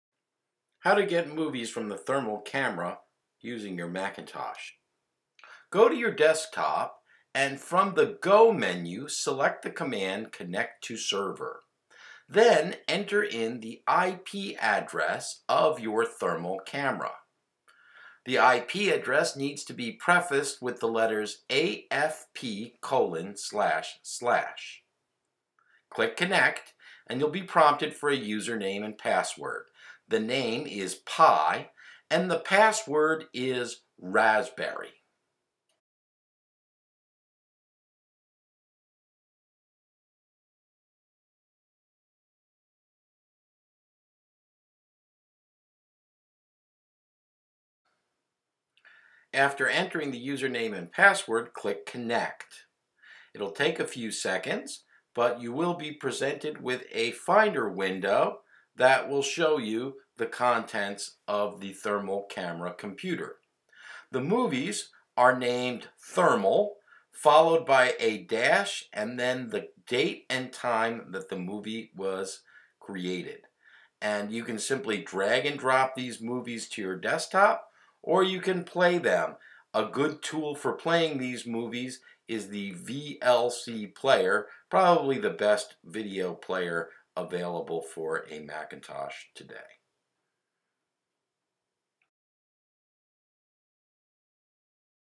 Get Movies From Raspberry Pi
How to copy the movies from the thermal camera onto your computer.